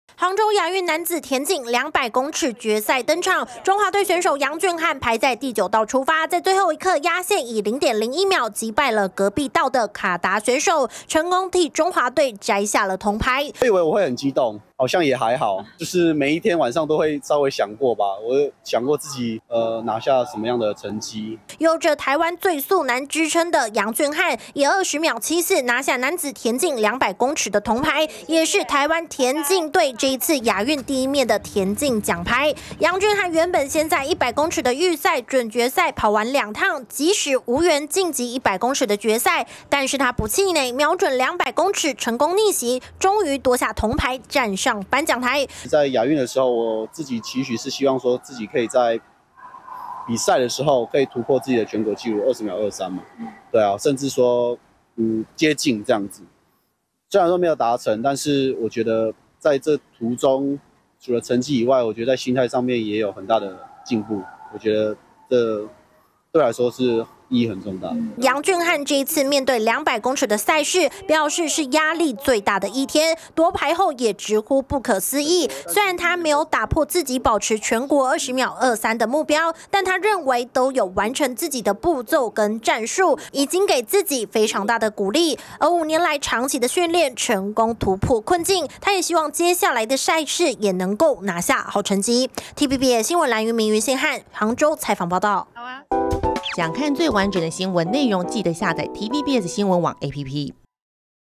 僅差0.01秒! 楊俊瀚摘亞運200公尺逆襲摘銅｜TVBS新聞
中華隊田徑好手楊俊瀚，在杭州亞運男子200公尺決賽，以20秒74拿下銅牌，僅以0.01秒擊敗隔壁道的卡達選手，也替台灣摘下這一屆亞運第一面田徑獎牌